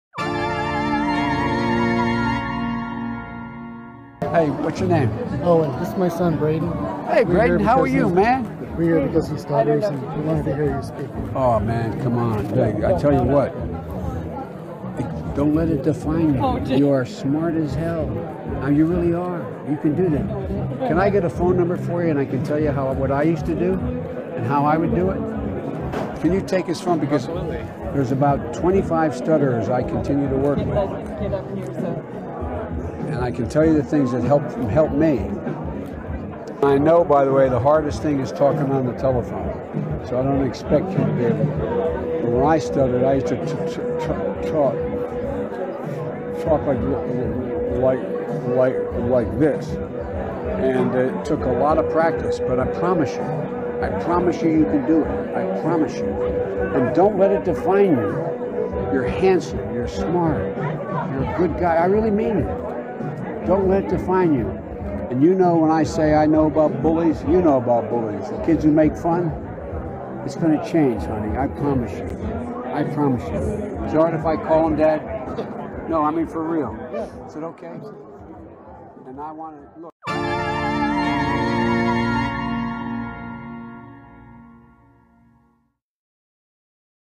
Beautiful: Biden & little boy who stutters (debunks rightwing lie)
...for those broken souls who think empathy, decency, kindness and overcoming challenges are creepy.